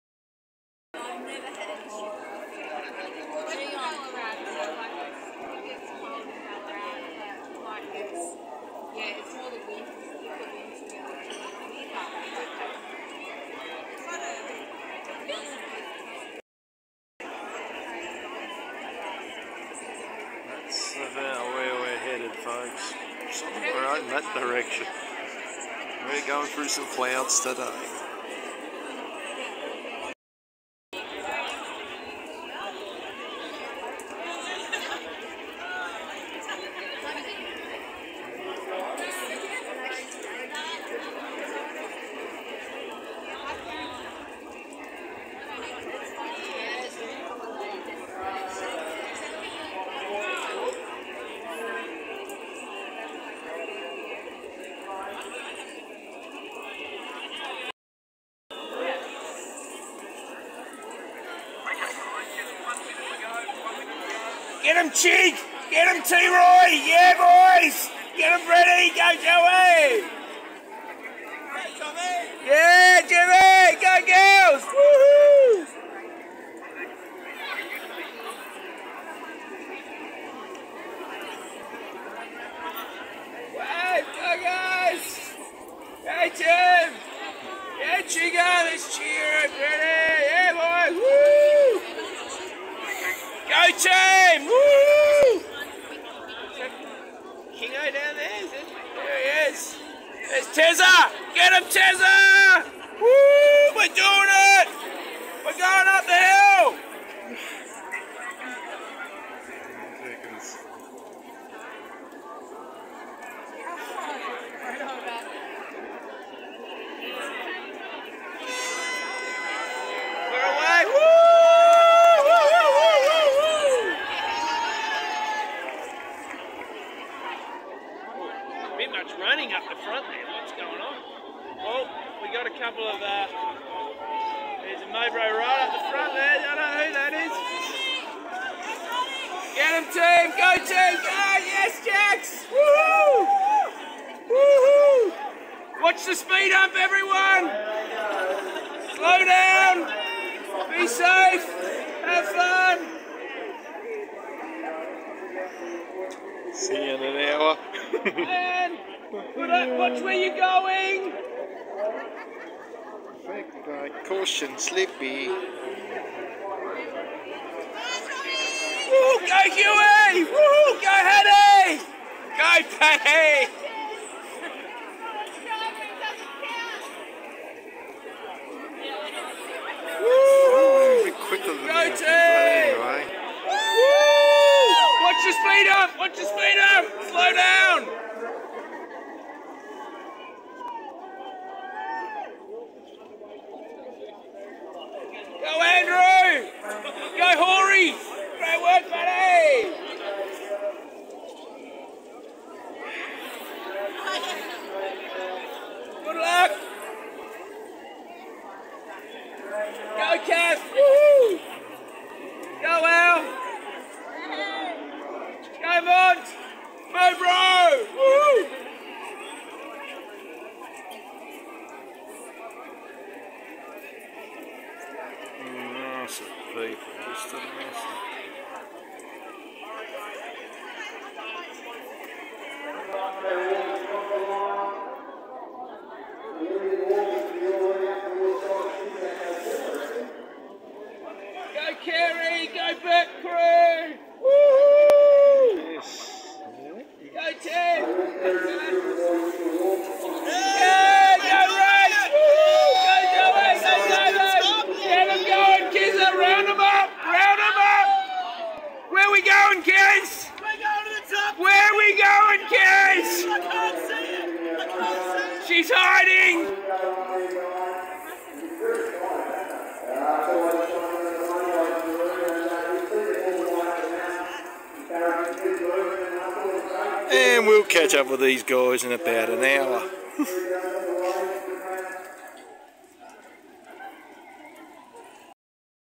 Point To Pinnacle 2022 WALKERS START
Runners will catch a lot of these in about an hour or so.